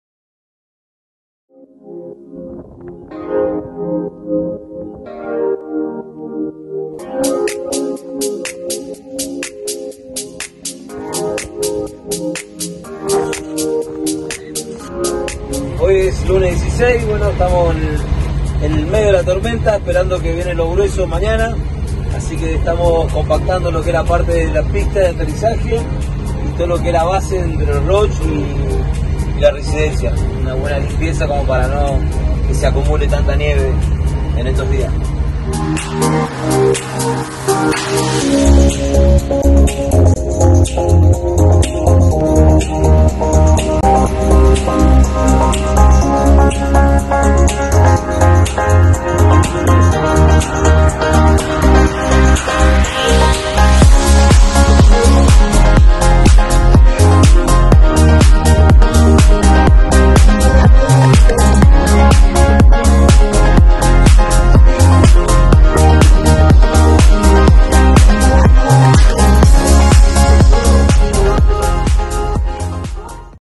Nevada en El Azufre, Malargüe, 16 de agosto de 2021
La nieve se hizo presente a la mitad de agosto en El Azufre, Malargüe. Nota en memo.com.ar